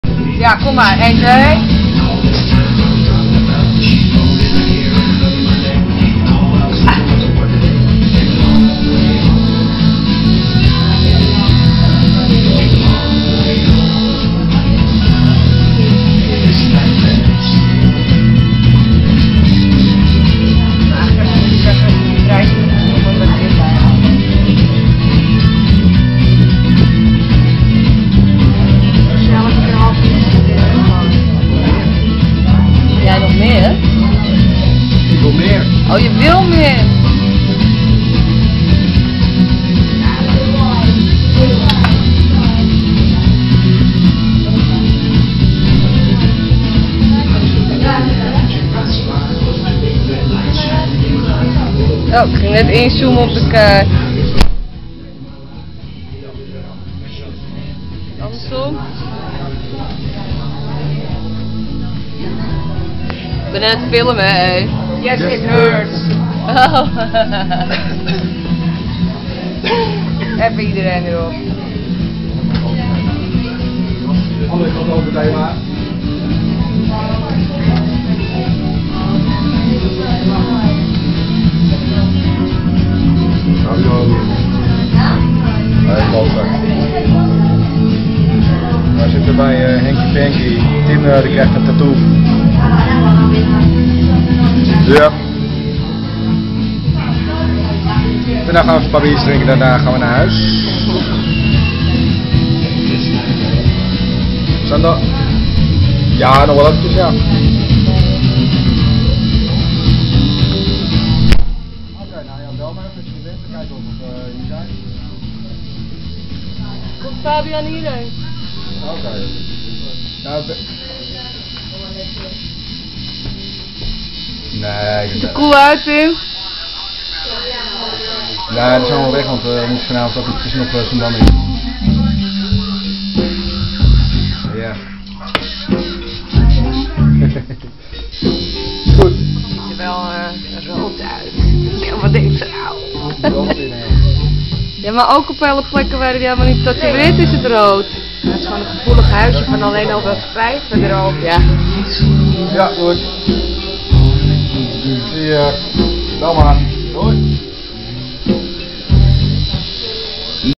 Tattoo Tim Live @ Hanky Panky
He's getting a tattoo, he's getting ink done..